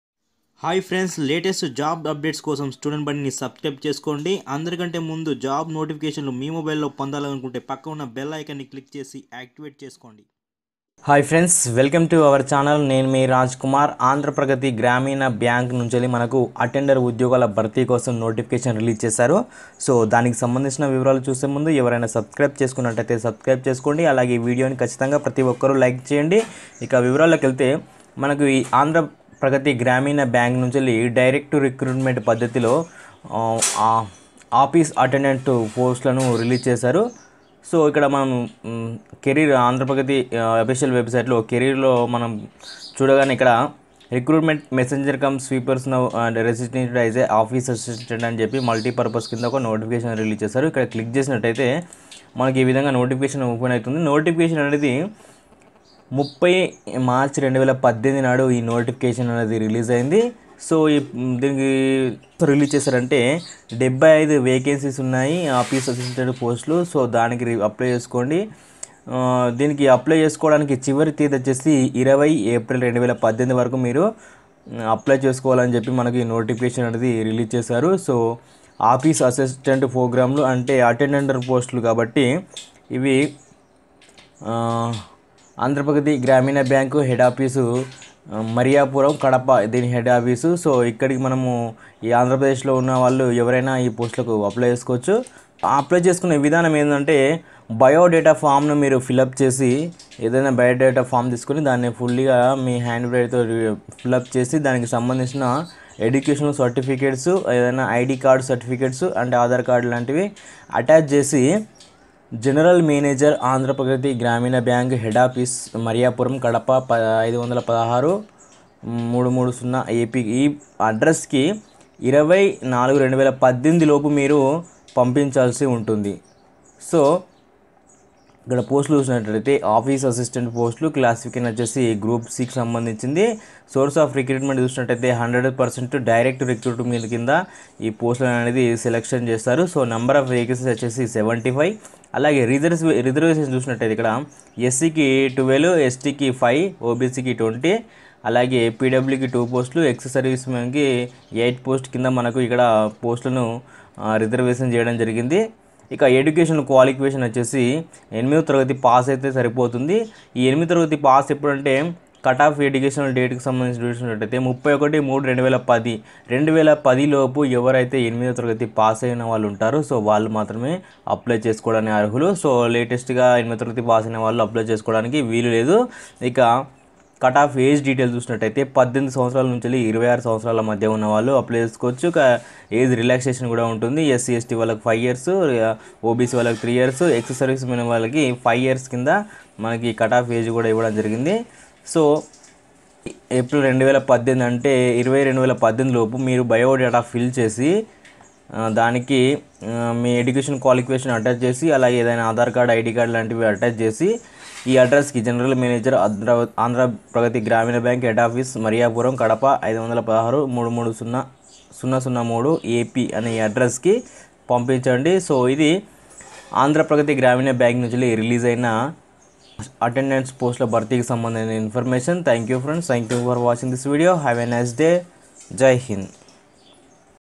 బ్యాంకు లో అటెండర్ ఉద్యోగాలు | Andhra Paragathi Gramina bank Office Attendant Recruitment 2018
ఎంపిక విధానం: ఇంట‌ర్వ్యూ ఆధారంగా
నోట్:
కడప, కర్నూలు, అనంతపురం, నెల్లూరు, ప్రకాశం జిల్లాల పరిధిలోని జిల్లా ఉపాధి కార్యాలయాలు, జిల్లా సైనిక్ వెల్ఫేర్ బోర్డులు ప్రతిపాదించిన అభ్యర్థులు; కోర్టు నుంచి తగిన ఆదేశాలు పొందినవారు మాత్రమే ఈ ఉద్యోగాలు దరఖాస్తు చేసుకోవడానికి అర్హులు. వీరికి వ్యక్తిగతంగా బ్యాంకు దరఖాస్తు పత్రాలను పంపింది. studentbadi, jobsupdatesintelugu, telanganajobs, andhrapradeshjobs, centraljobs studentbadi